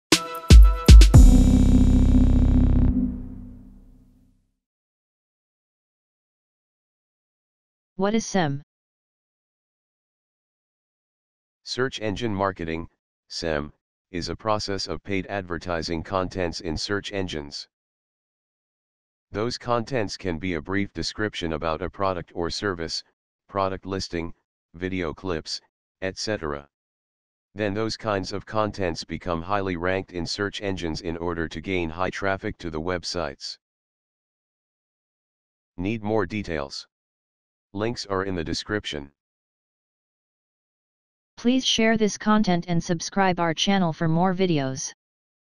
SEM
This video purpose is to provide brief explanation  of SEM (Search Engine Marketing).
If  you need to know more bout this topic use this links for further references.

Hope you all got idea about SEM.